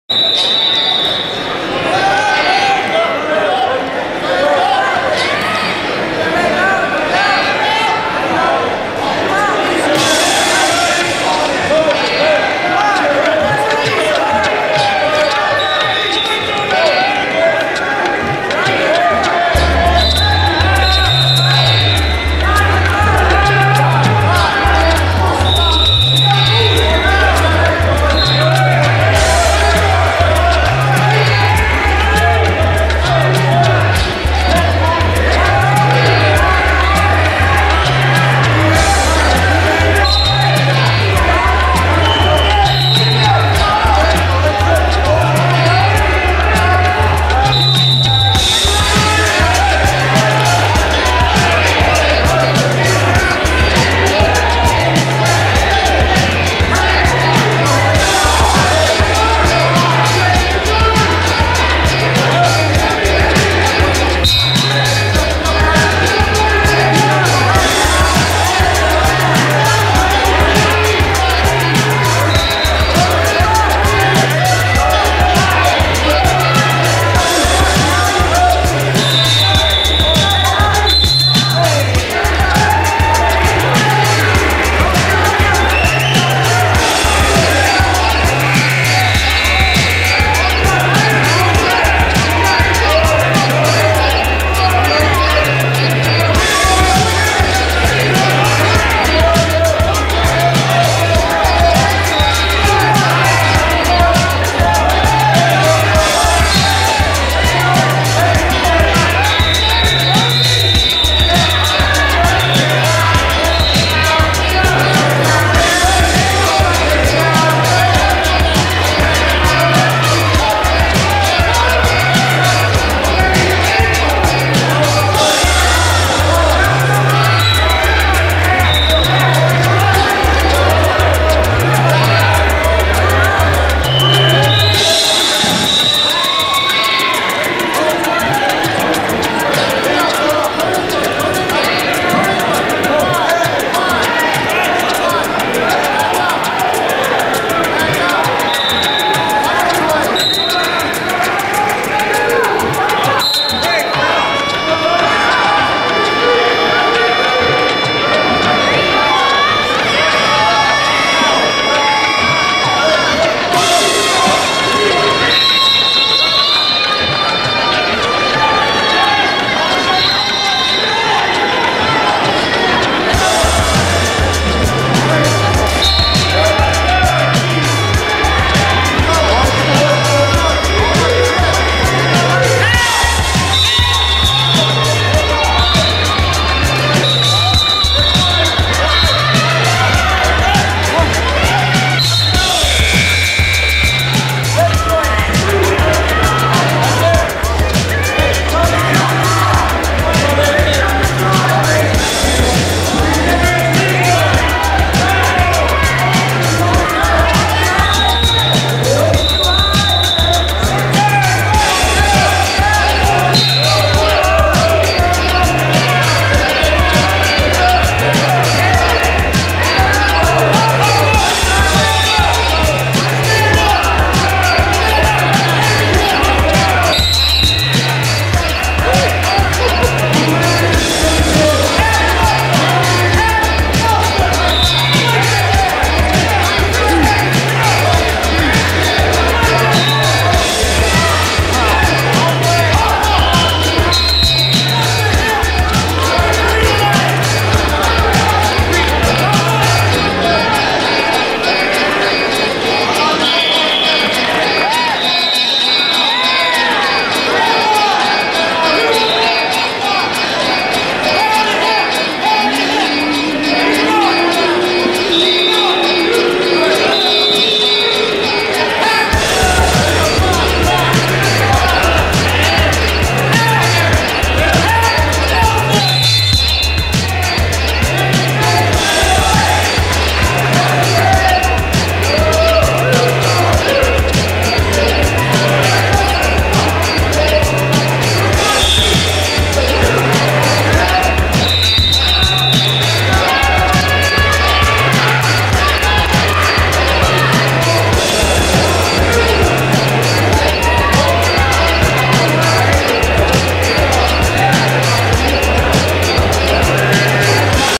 Conner Popham Wins match 16-0
Fox Jagwarriors' Conner Popham wins at Officials Tournament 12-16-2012